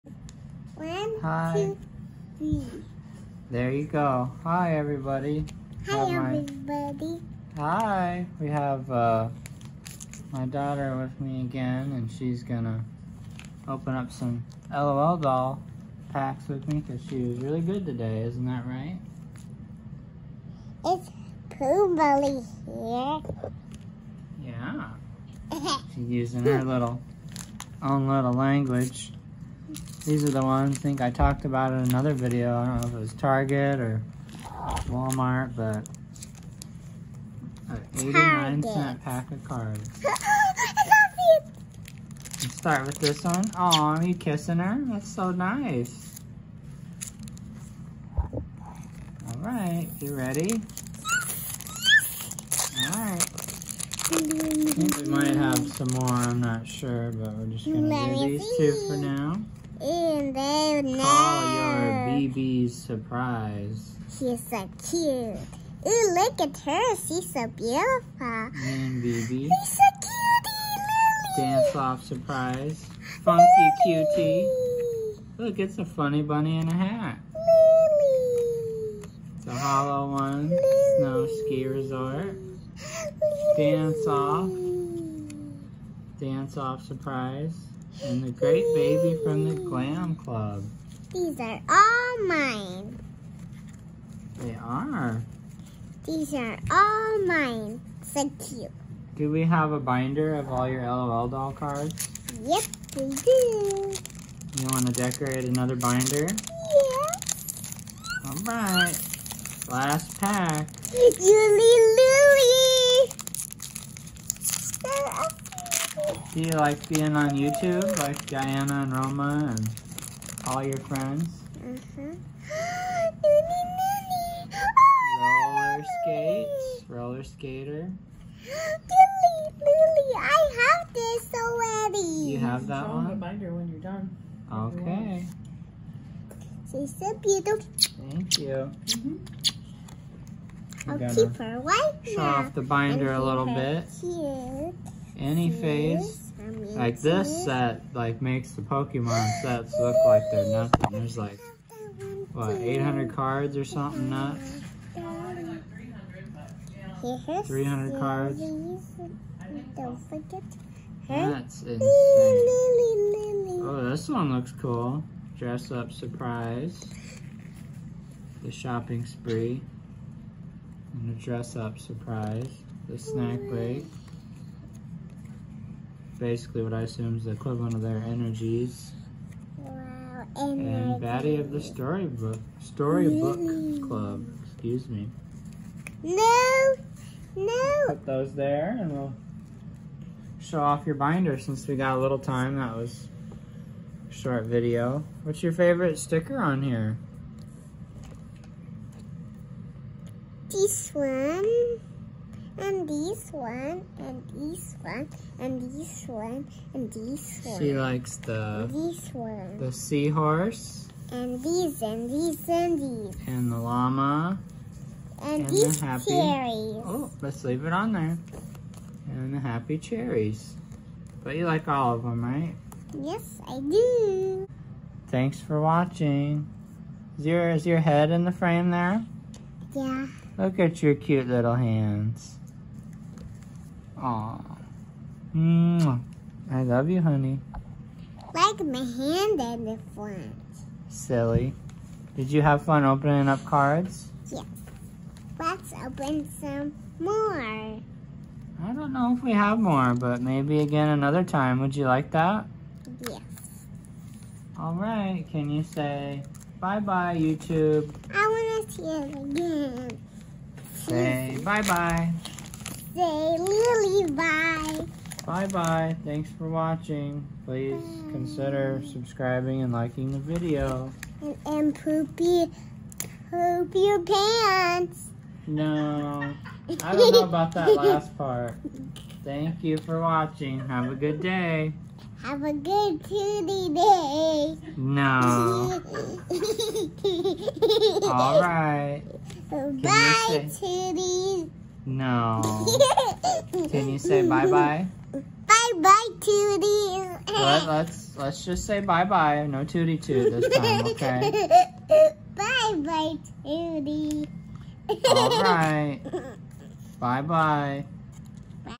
LOL Surprise Doll Card Dance off with my daughter
Opening 2 packs of LoL surprise doll cards that my daughter collects. Nice way to get kids to start a special collection of their own!